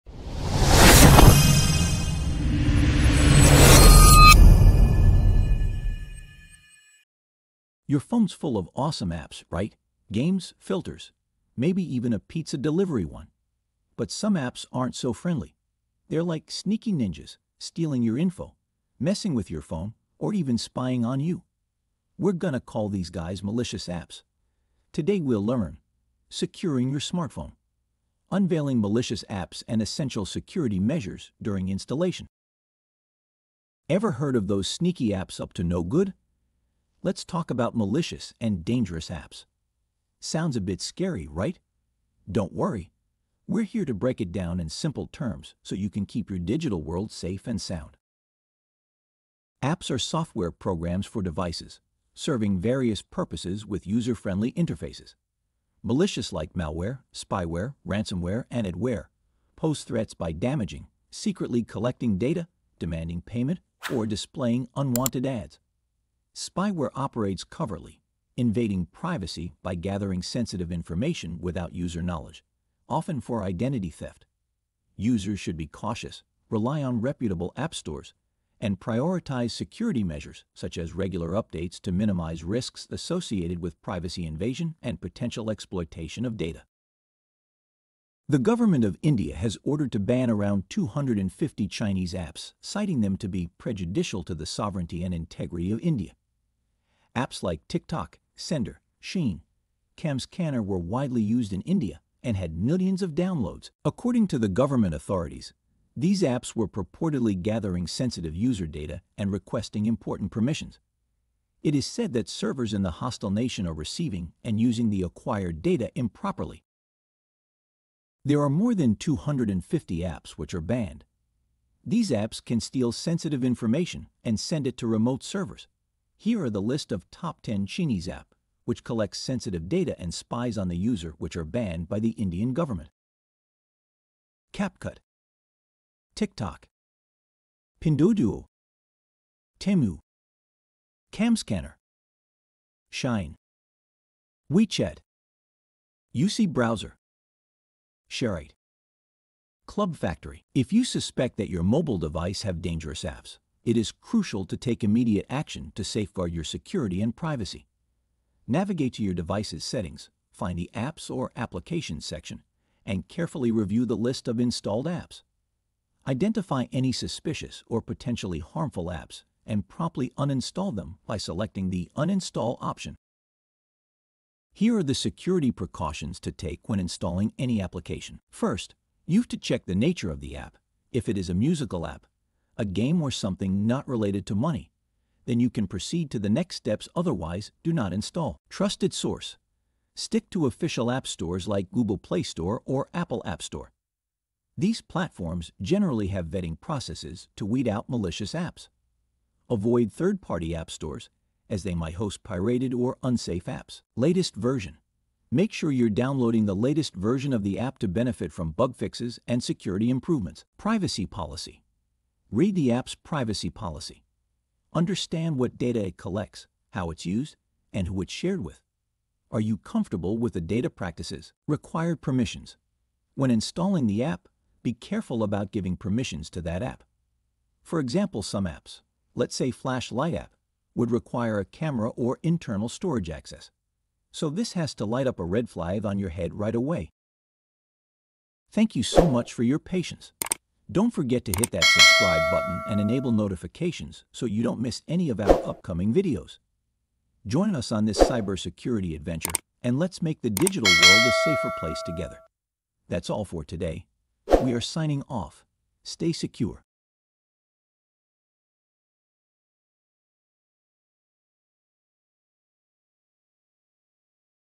Beware of these Chinees Apps ⚠️⚠️
In the world of apps, including those from China, it's wise to stay alert. Some apps might raise security concerns, so take a moment to check permissions and be aware of potential risks before hitting download. Keeping your digital safety in mind ensures a secure and worry-free experience with Chinese apps and beyond.